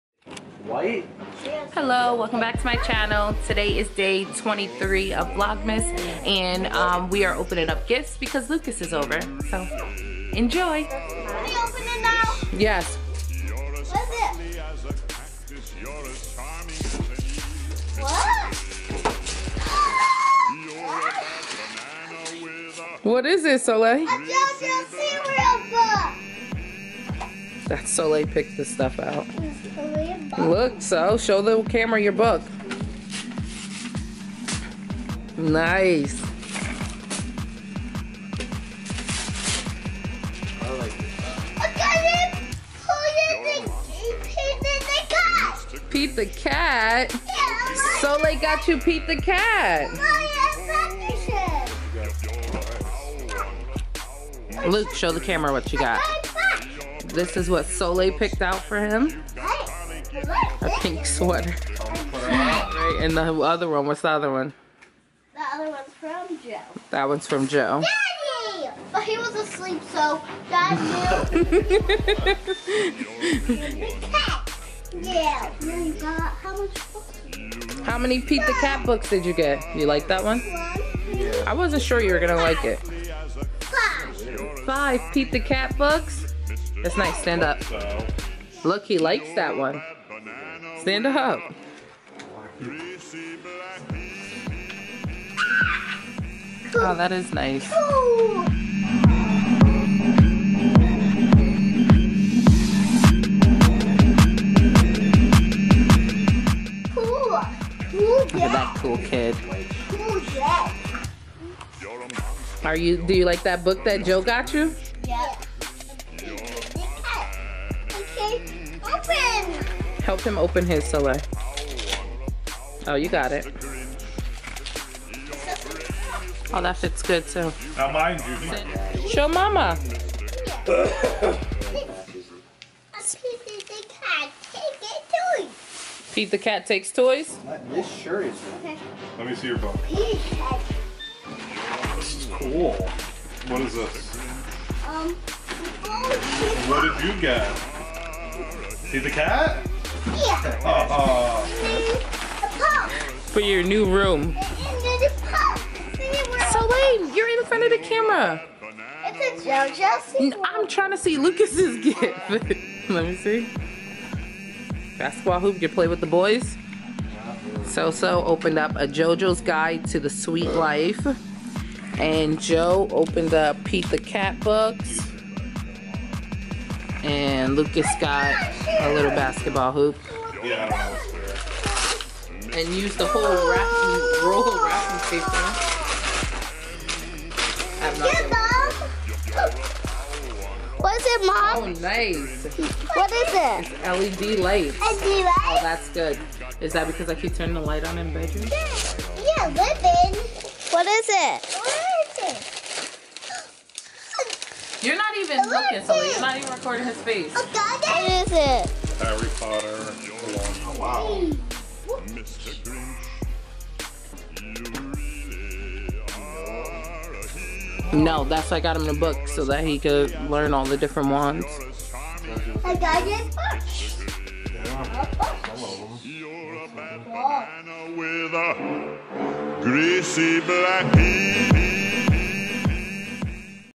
Opening Gifts Early | Vlogmas Day 23 | The Busiest Bree
Hello and Welcome to VLOGMAS 2017, DAY 23!!

Lucas is with us for the night so we decided to open gifts with him just incase we don’t see him on Christmas. Who knew that SoSo had an eye for fashion 😉

The Busiest Bree